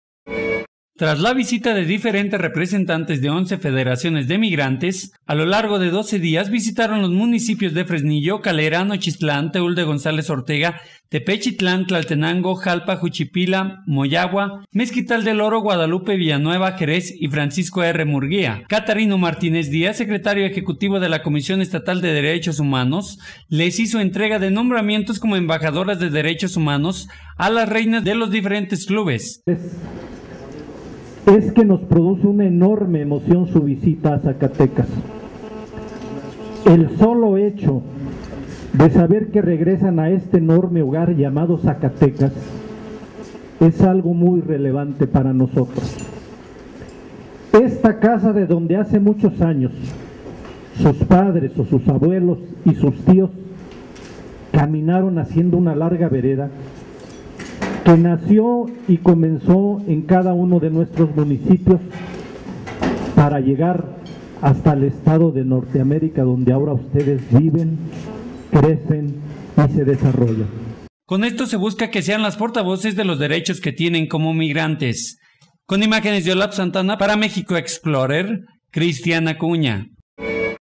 CEDH entrega nombramientos como embajadoras de derechos humanos a las reinas de los diferentes club
Tras la visita de diferentes representantes de 11 federaciones de migrantes, a lo largo de 12 días visitarán los municipios de Fresnillo, Calera, Nochistlán, Teúl de González Ortega, Tepechitlán, Tlaltenango, Jalpa, Juchipila, Moyahua, Mezquital del Oro, Guadalupe, Villanueva, Jerez y Francisco R. Murguía,  Catarino Martínez Díaz Secretario ejecutivo de la comisión Estatal de derechos humanos les hizo entrega de nombramientos como embajadoras de derechos humanos a las reinas de los diferentes clubes.